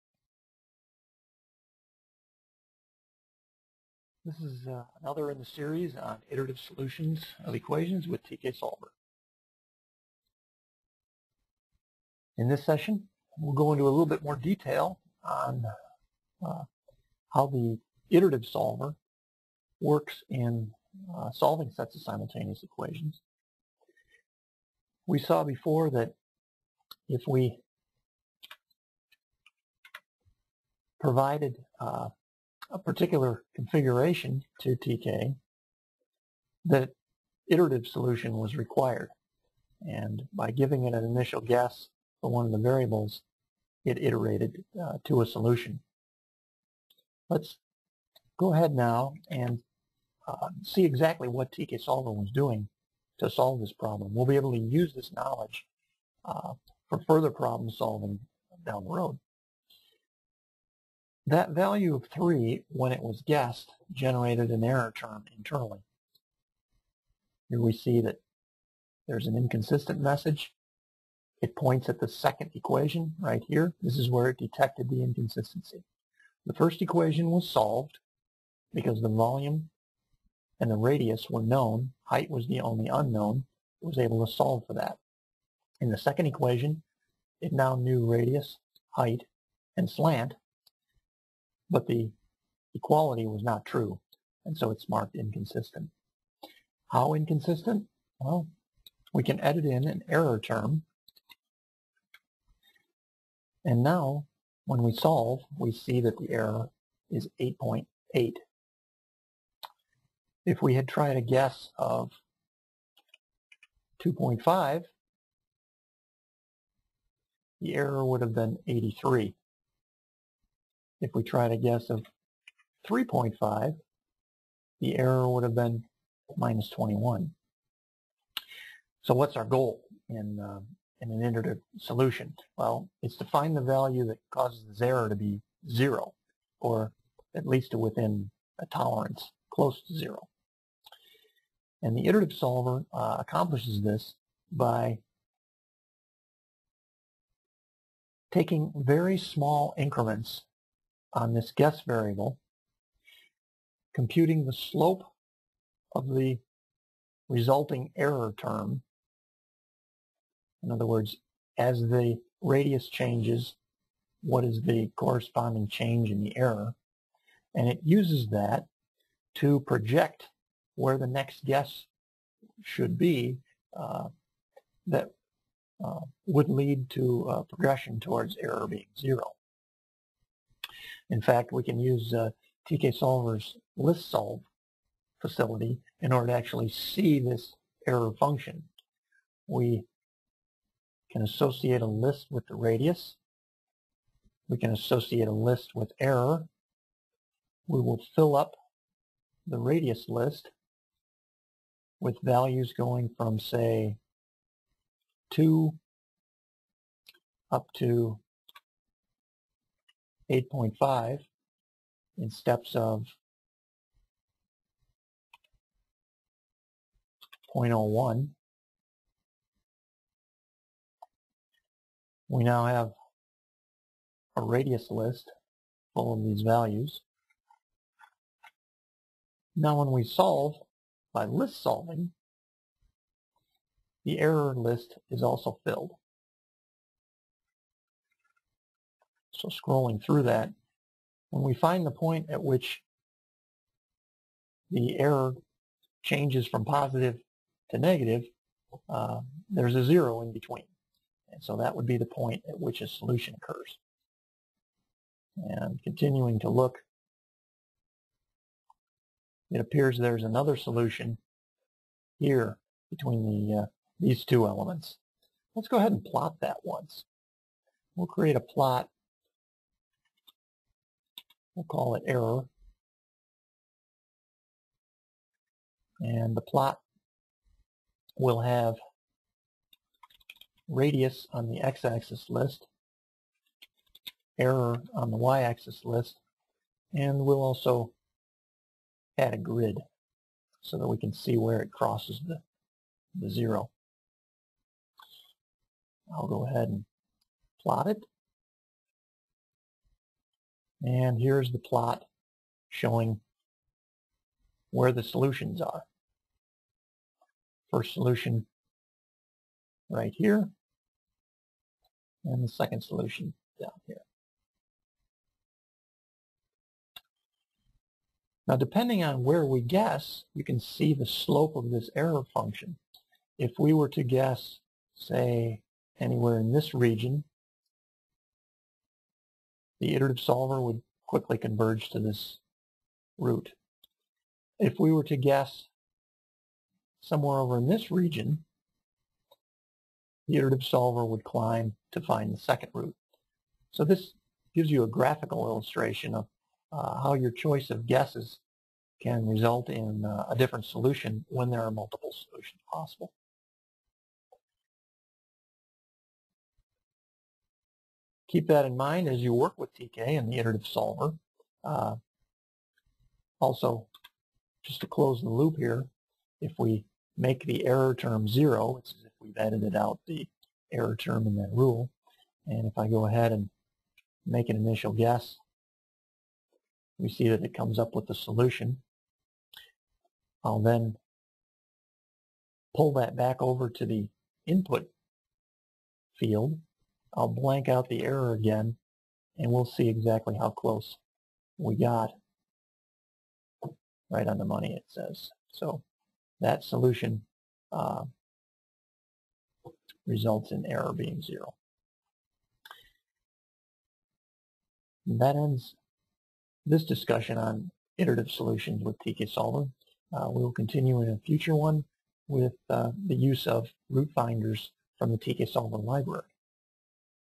TK Solver - Iterative Solutions 2
This series covers the built-in Iterative Solver, Optimizer, and Root-Finders from the TK Solver Library.